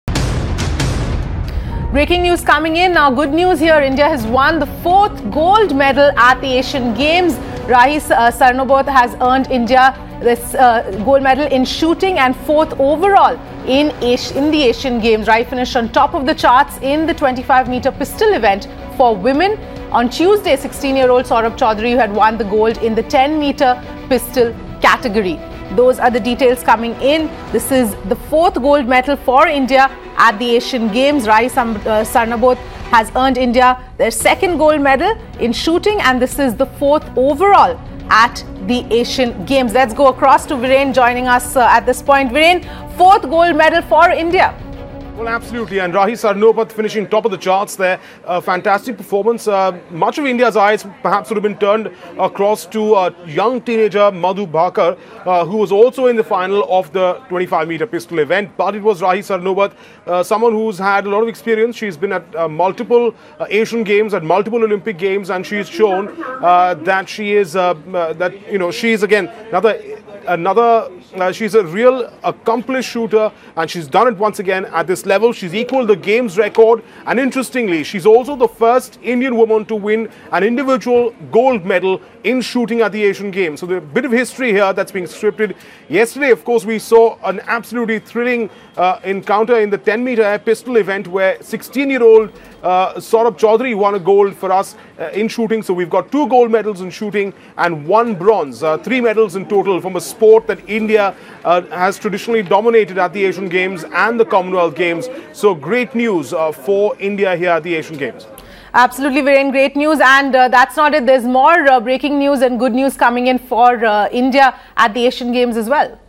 Indian Shooter Rahi Sarnobat Bags Gold In Women's 25 M Pistol Event At Asian Games 2018
Rahi Sanobat wins gold for India in women’s 25 M pistol event at the Asian Games 2018. She is also the first Indian woman to win individual gold shooting in the history of India.